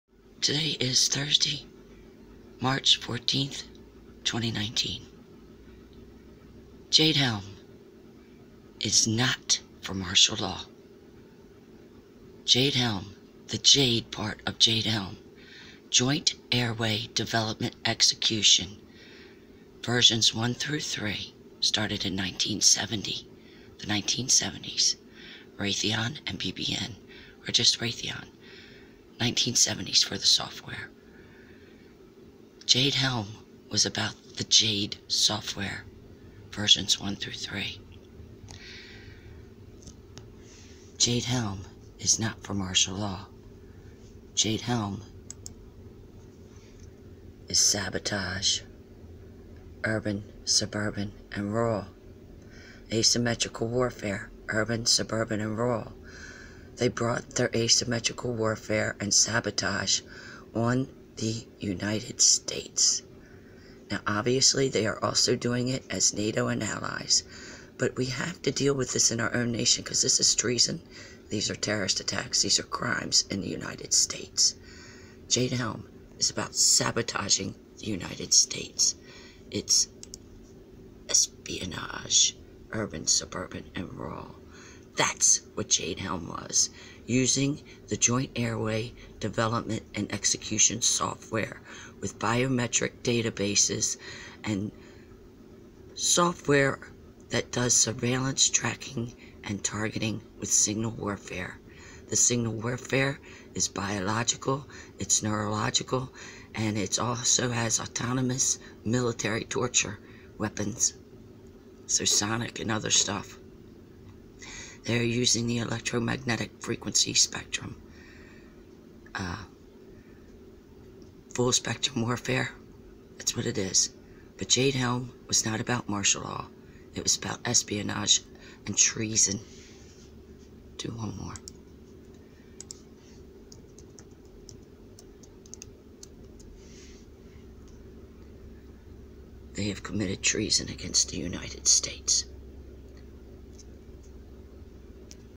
JADE Helm NOT Martial Law: Subversion, Sabotage, Espionage: Urban Suburban Rural = TREASON
To be used as Evidence against sadistic inhumane criminals, home grown terrorists, &  traitors of the USA in the next Nuremberg Trails for DHS + American & Ally Nazi wanna bes.
 Charges: Inhumane Crimes against Humanity. Threats to World Peace, Terrorism, TREASON.
Appeal for help exposing serious inhumane crimes with ILLEGAL 1033/Military weapons on Innocent Citizens in Society
Evidence on hard drives, acoustic devices & mobile communications at ALL levels.
Local, State, Federal & Armed Forces
Either as Fusion Center/FEMA/DHS access or direct on devices, servers, computers

Radio waves (RF) ElectroMagnetics (EMF) Full Spectrum warfare
Acoustic devices & Mobile Communications as a weapon at certain settings
AI Autonomous Domain Awareness Weapon Systems with Deployment Planners
Real People, Real World Simulators
AGI Artificial General Information (IBM Watson, Google Deep Mind)
Intelligence Agency/NSA personal data information (personal profiles)
Biometric Targeting Recognition as Human GPS Coordinates (DoD)
Location Geo Spatial Targeting (using antennas, radars, satellites from land, sea & air)
Covert Neurological warfare is from illegal MKUltra still ongoing
MKULTRA is the NEUROWEAPON - automated, computerized AI autonomous weapon

Plus there's Covert Biological Warfare - signal transmissions that mimic, imitate REAL illness, disease, injury even to death. Worse - you only die when they or computer kills you natural or not.
Using GITMO Electrocution type of torture - includes Pulsated Electrical attacks & continuous streams
Personalized Deconstruction, Depatterning, Dehumanizing, Desensitizing & Sensitizing,  Repetition,
Add, Delete, Manipulation Brainwashing
Dark/Sick/Vulgar/Perverse/Violent Psychology
Live and/or recorded/automated auditory torture

Land, Sea, Air - Domestic & Foreign
Mastering the Human Domain & Terrain
J.A.D.E. AI at the "helm", running the "show" we call Life
Real People, Real Impacts, Real Lives

Covert signal transmissions -
Delgado, Nazi Mengele, Sidney Gottlieb, Ewen Cameron, CIA Human Command & Control
(acts just like BCI is described but takes out real neurology, replaced w/augmented version)
includes Michael Persinger's effect - automated, computerized
It can be Detrimental
It can be Normal
It can be Genius
It is On Demand Conditions permanent or temporary
It can seem neurological
It can seem psychological
It can seem biological
It can have mental type of HDMI/VFX personalized & general info visuals (retinas - eyes, temples, back of skull @ base of neck) (very vivid & detailed, "easily" recalled/verbatim)
It can be like Synthetic Telepathy or work like Subliminal Suggestion  (inner ear/cochlea)
In the most hideous inhumane application:
It can stop you from speaking or make one babble incoherent
It can stop you from crying or make one cry
It can stop you from moving, cause erratic movements/reflexes or control your movements
It can stop you from screaming or make you scream
It can render a person conscious or unconscious
It can stop you from defending yourself
NEUROWEAPON is the MOST DANGEROUS WEAPON ON EARTH.
You can't see this weapon with the naked eye.
11/5/2020